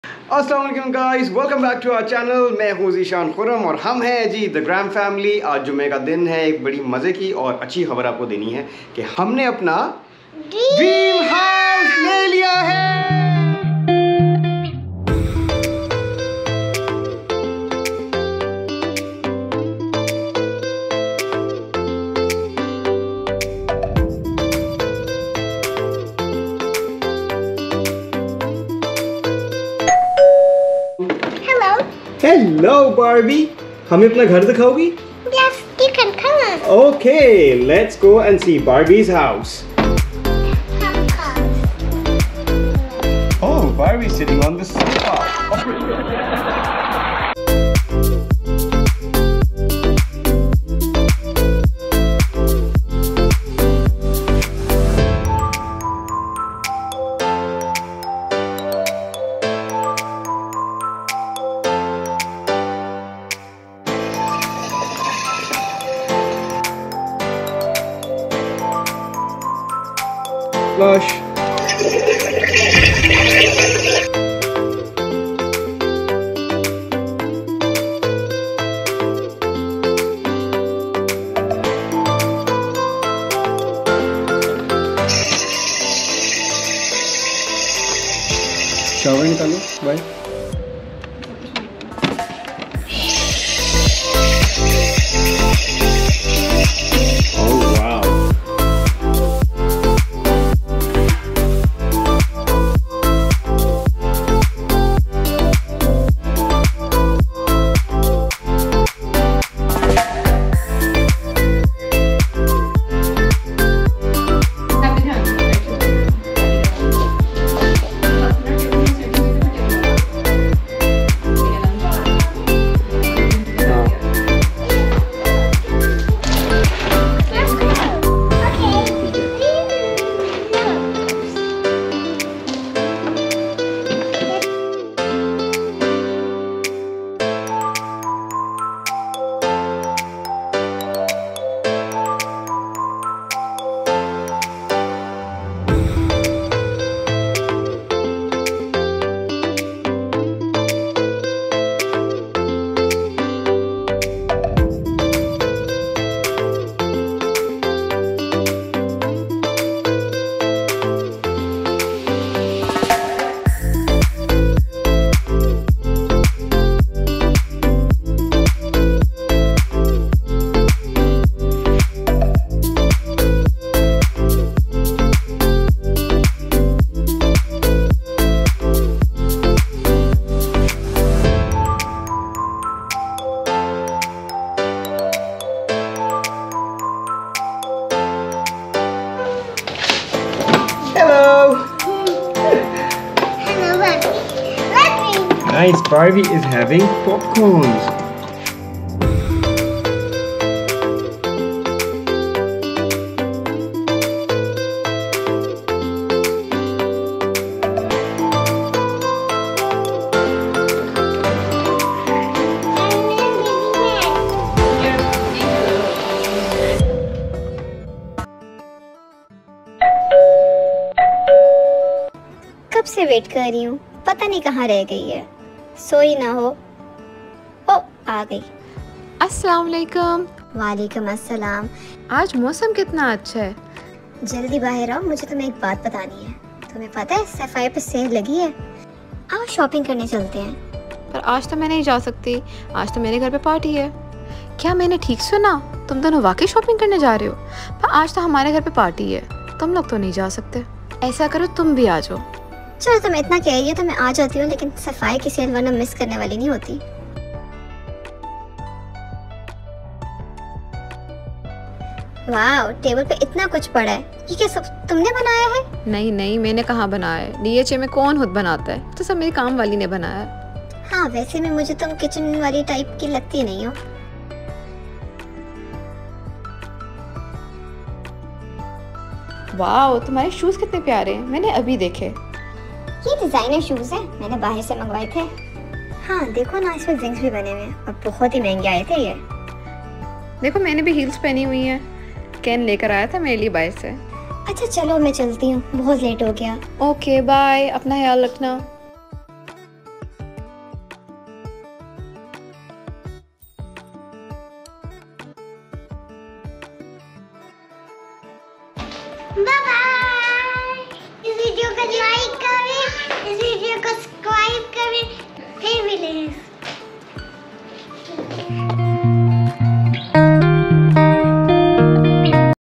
We bought our Dream House | My Little Barbie Hosted A Party
Watch the video to see the new house and enjoy the little Barbie voice over.

We got this Barbie Dream House from Entertainer in Pakistan. They will import it for you.

Price ranges between 85K to 95K depends on the USD exchange rate so best to check both the stores before buying.

Musician: @iksonmusic

Musician: @iksonmusic

We want to create content that you enjoy which is why we would love your input in the comments.